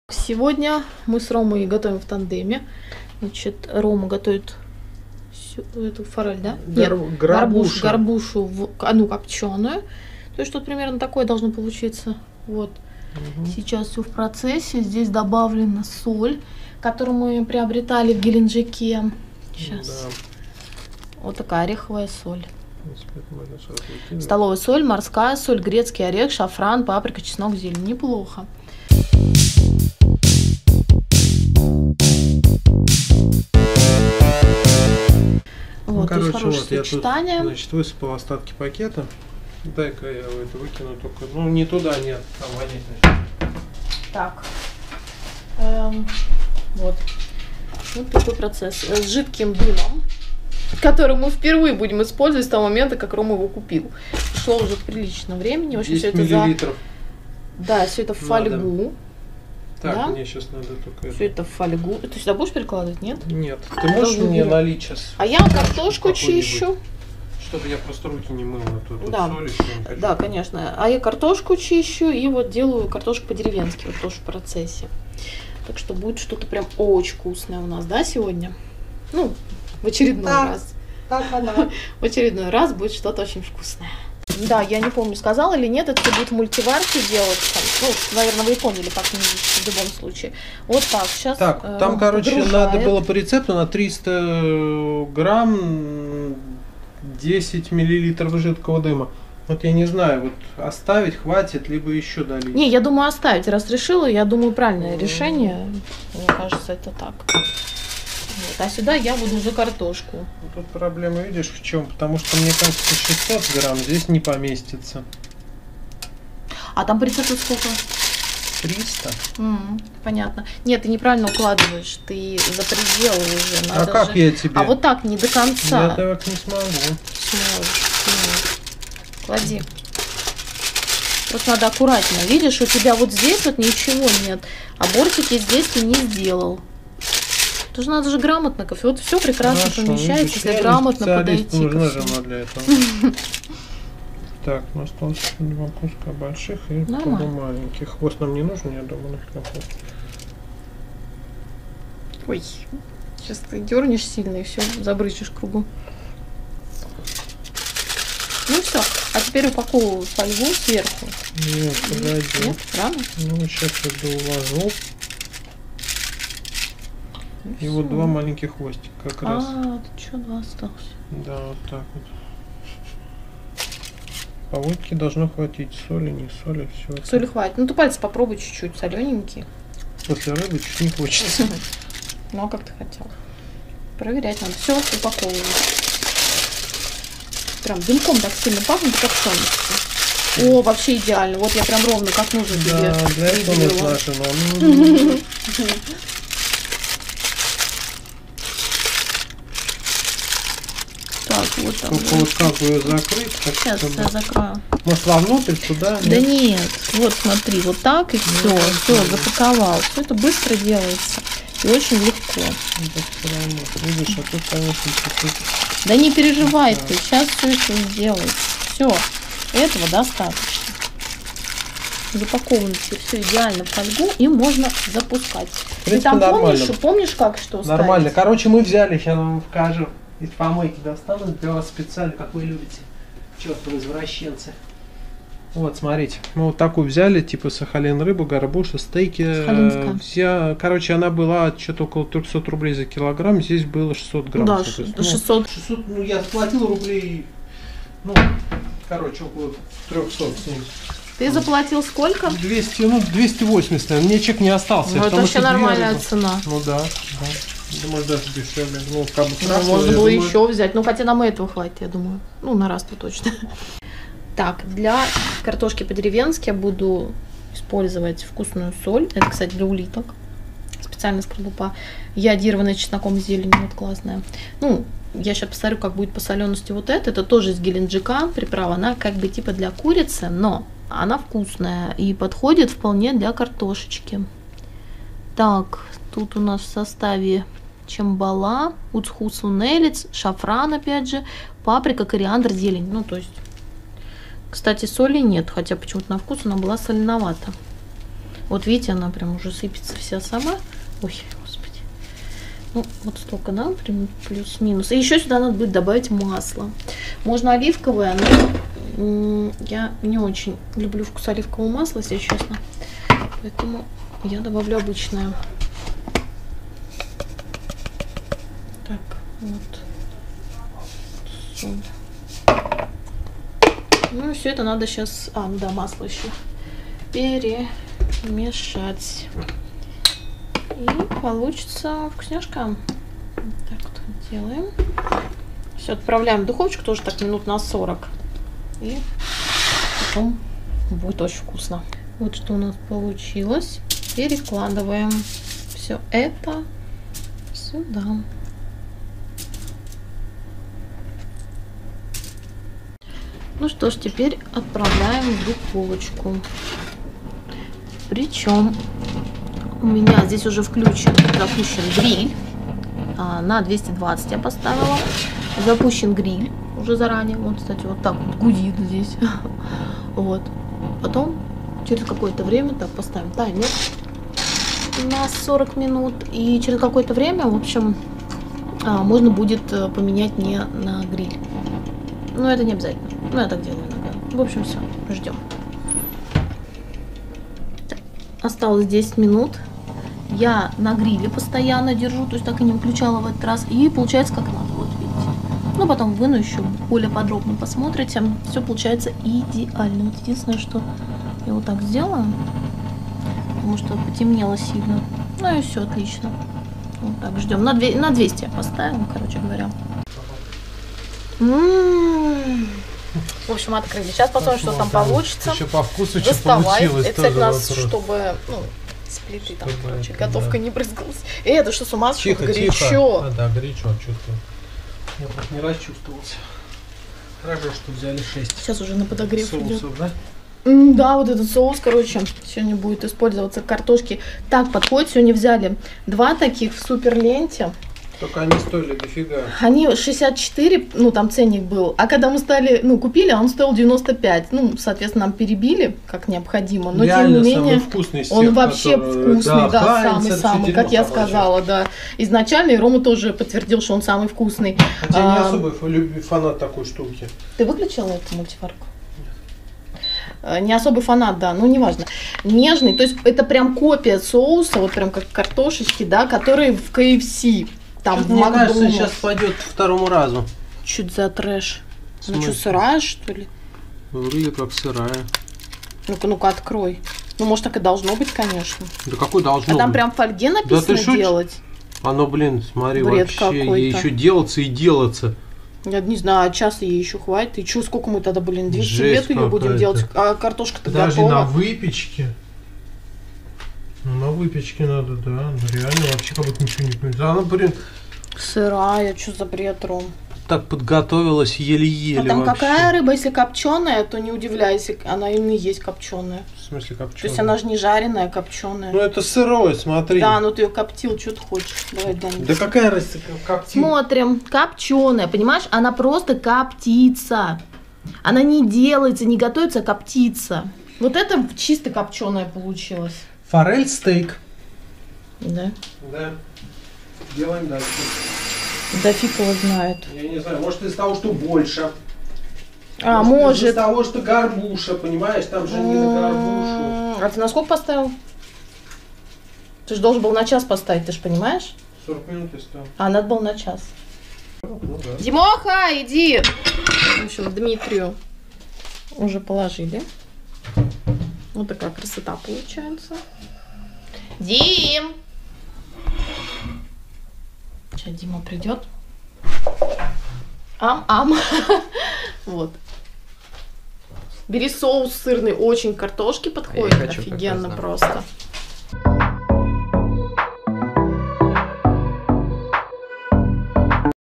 Горбуша с дымком и картошка по - деревенски. Рецепт / Готовят муж с женой
Here you're always welcome !!! ❤❤❤